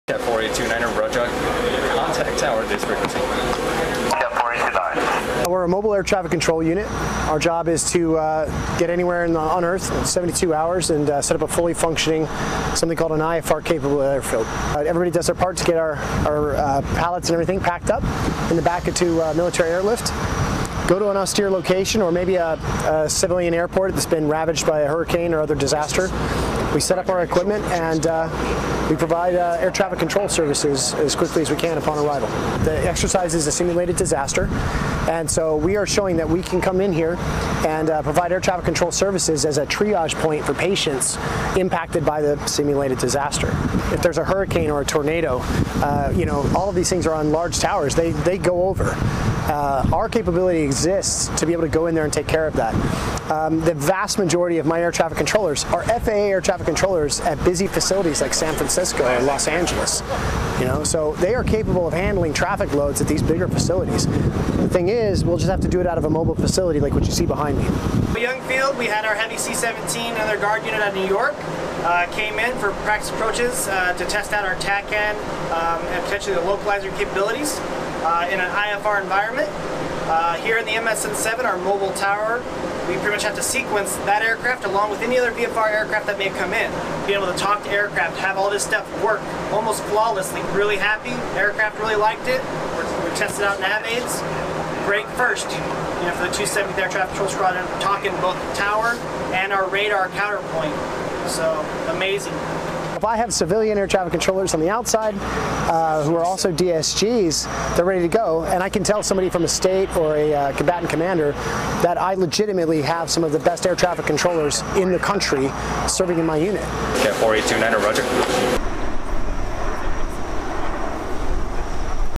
Mobile Air Traffic Control Training at PATRIOT 21
VOLK FIELD AIR NATIONAL GUARD BASE, WI, UNITED STATES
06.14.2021

Air National Guard members from the 270th Air Traffic Control Squadron trained with a mobile air traffic control system (ATCS) designed to operate in a degraded environment like the one simulated during PATRIOT 21 at Fort McCoy, Wis., June 14-17, 2021. PATRIOT 21 is a training exercise simulating an earthquake, and is designed for civilian emergency management and responders to work with military entities in the same manner that they would during disasters.

Film Credits: Video by Chief Master Sgt. David Lipp
119th Wing Public Affairs North Dakota Air National Guard

ANG
National Guard
ATC
PATRIOTNS
PATRIOT2021
PATRIOT21